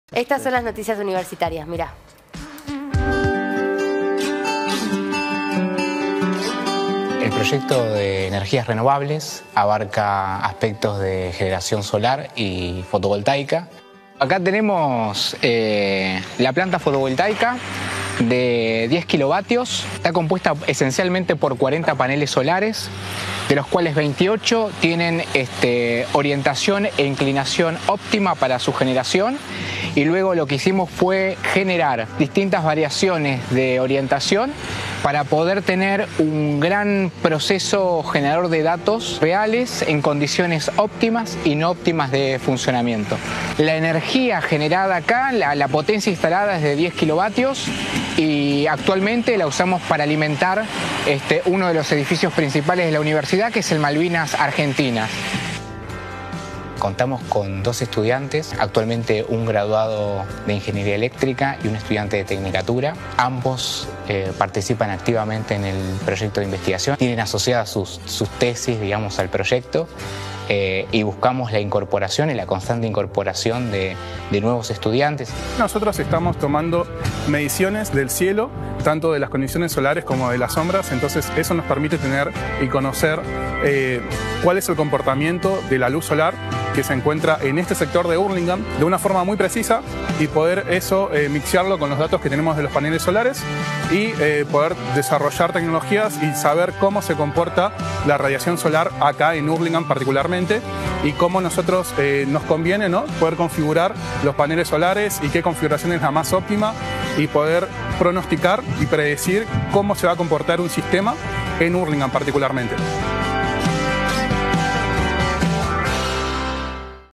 Energía renovable en UNAHUR - Desde el Conocimiento
La Universidad Nacional de Hurlingham cuenta con diversos proyectos de investigación en los que se involucran este tipo de energías. Entre ellos, se encuentra el proyecto de “Implementación de una planta piloto multipropósito de generación solar fotovoltaica con una completa integración de las variables eléctricas y meteorológicas locales para el estudio de simuladores, la promoción y el desarrollo regional” que trabaja con energía solar. El mismo posee una serie de paneles ubicados dentro del predio ubicado sobre la calle Teniente Origone al 151. Este sistema de paneles solares brinda energía a una parte del edificio Malvinas Argentinas, reduciendo de esa manera su consumo eléctrico. Según el Ing. Maximiliano Véliz, director del proyecto, «esta planta fotovoltaica tiene una potencia de 10 kW y está compuesta esencialmente por 40 paneles solares, de los cuales 28 tienen orientación e inclinación óptima para su generación».